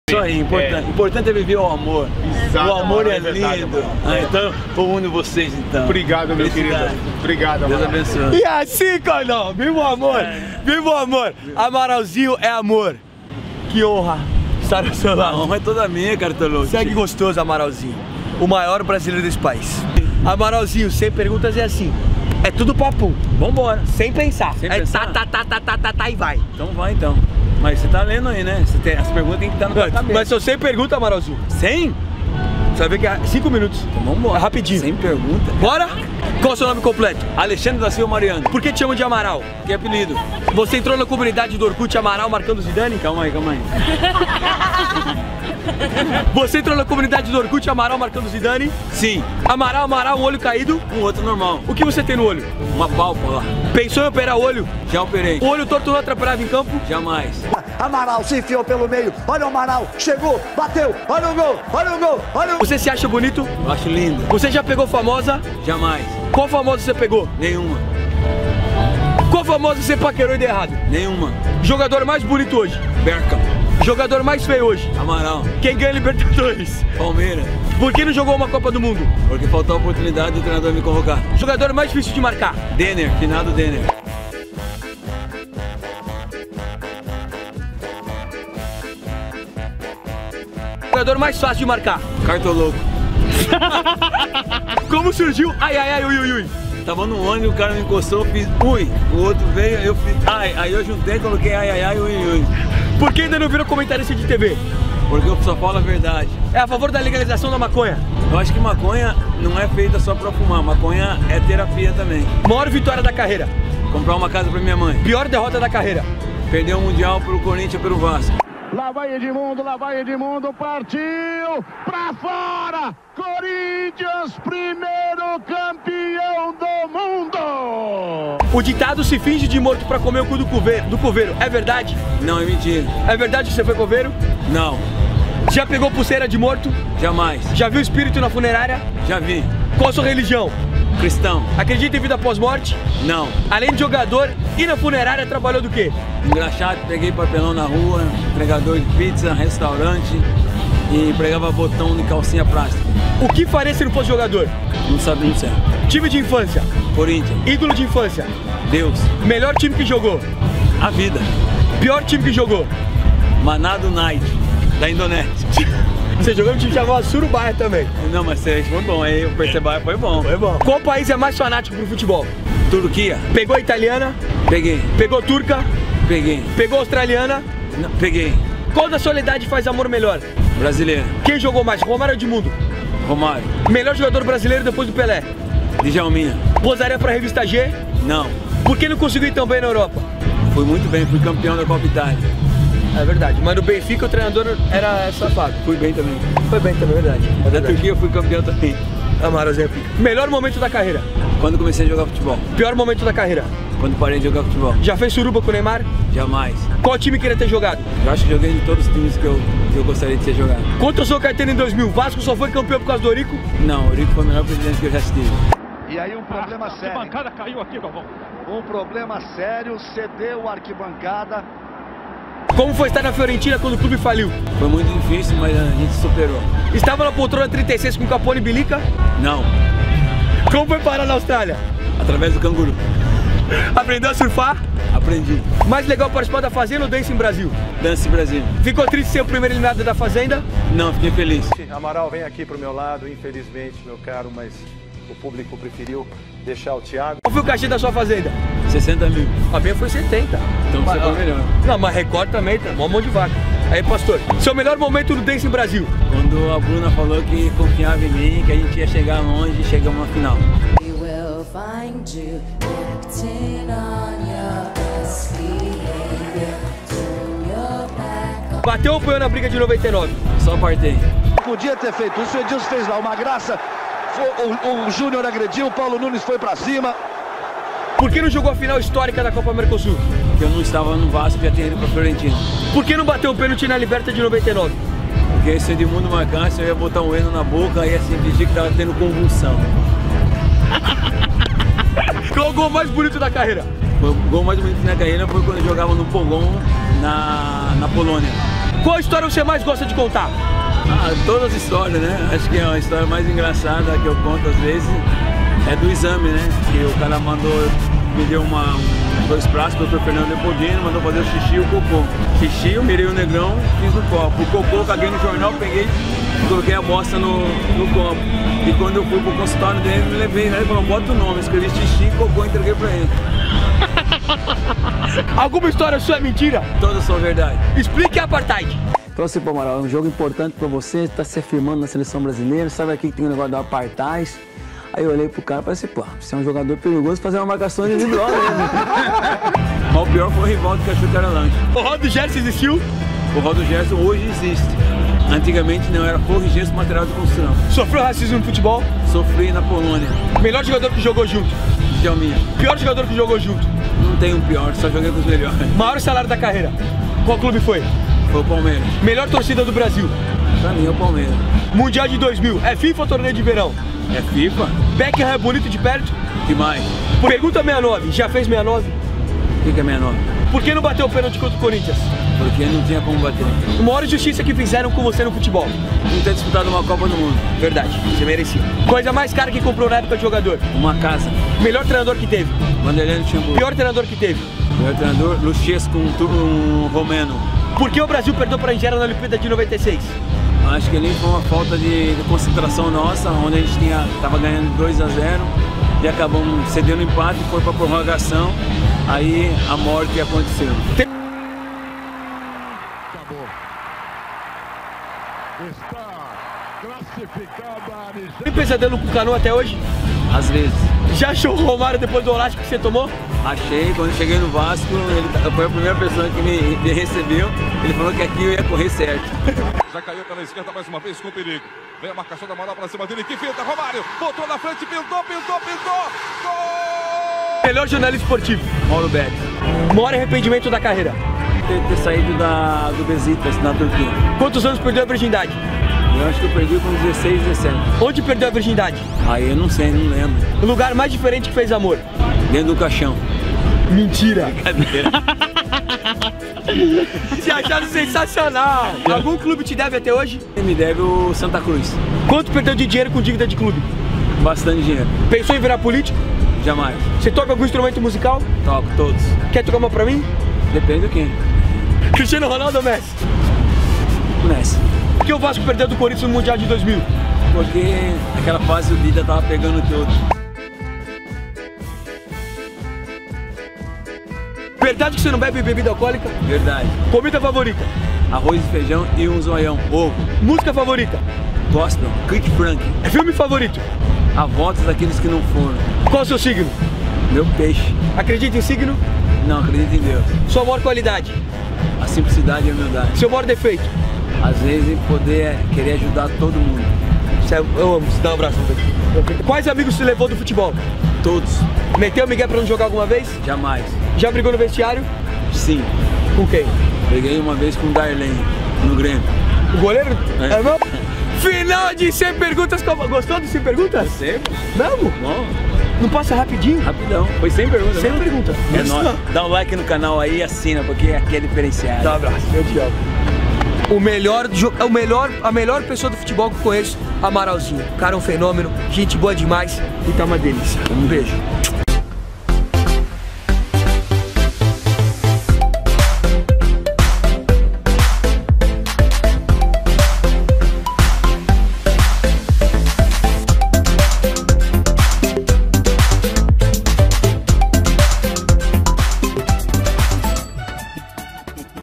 JÁ FIZ AMOR NO CAIXÃO - AMARAL NO 100 PERGUNTAS
Sexo no caixão? Prefiro o Palmeiras? Qual famosa já pegou? Já brigou no vestiário?
Só vem que esse 100 perguntas está lindo!
Aproveitei e deixa aqui seu like e comentário.